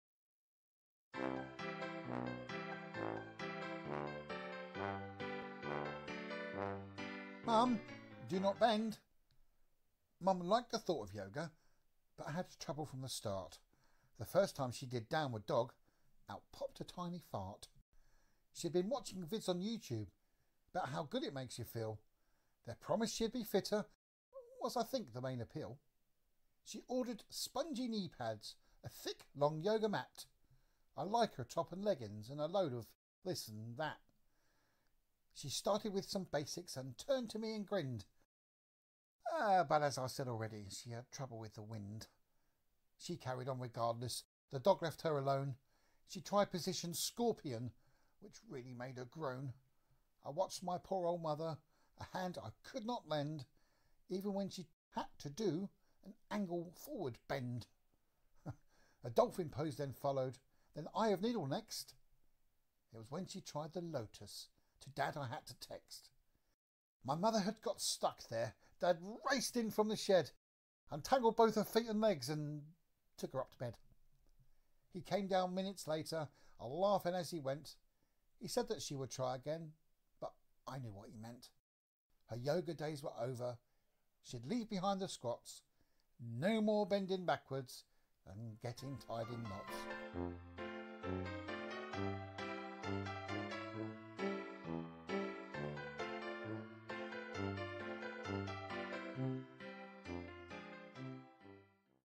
Mum - do not bend
My Mum wasn't as bendy as she thought